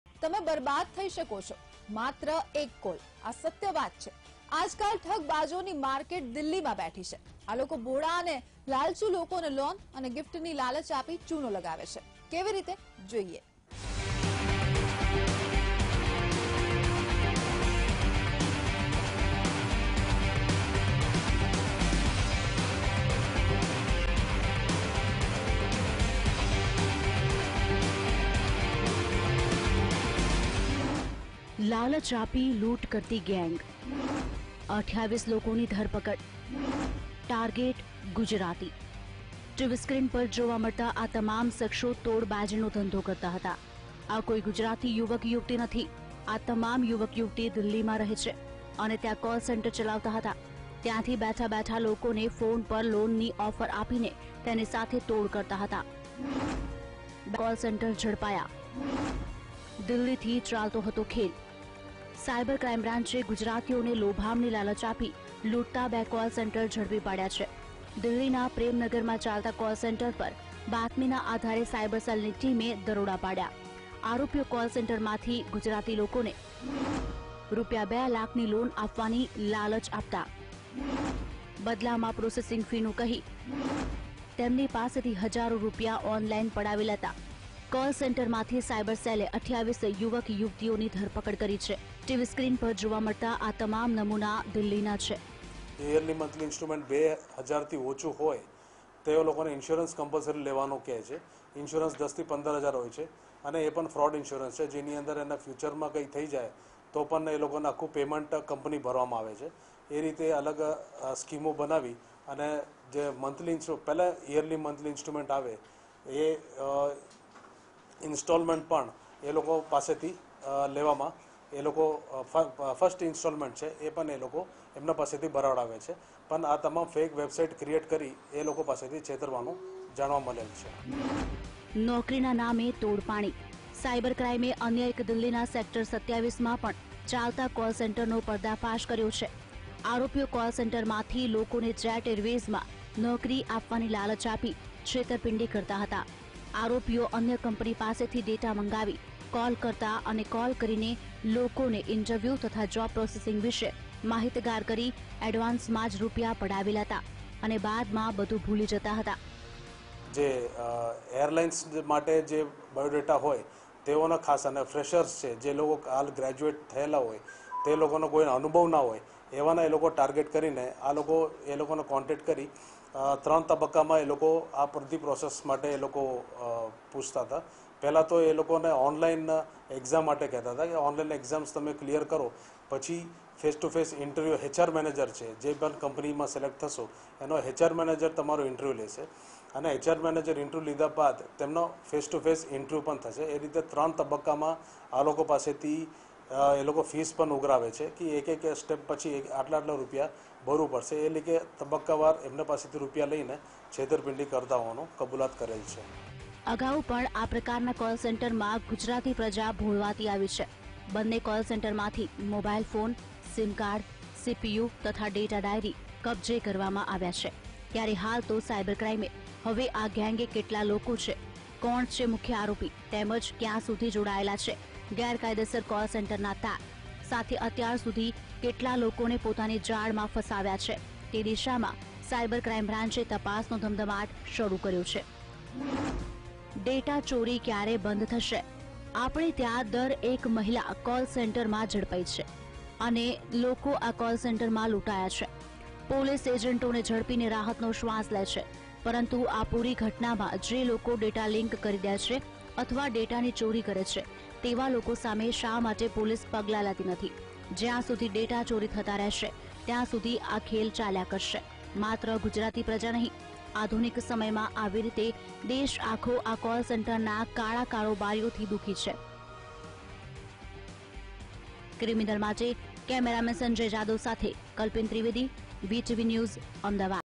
Ahmedabad Crime: Cyber Cell busted Fake Call Center in Delhi | Vtv News
Ahmedabad Crime: Cyber Cell busted Fake Call Center in Delhi